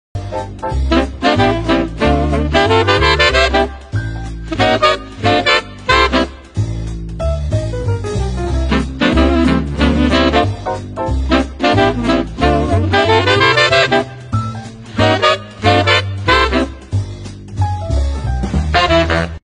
Expensive Michelin Chicken Cooked in Pig's Bladder
Famous Chef Reinhardt Gilles masters the art of michelinstar ‘en vessie’ 🐓

A traditional, timeless French cuisine from the 18th century whereby chicken is cooked within a pig’s bladder - flavored with truffle, foie gras, Madeira wine and other delicacies 😍

📍 Paul Bocuse - Le Restaurant Gastronomique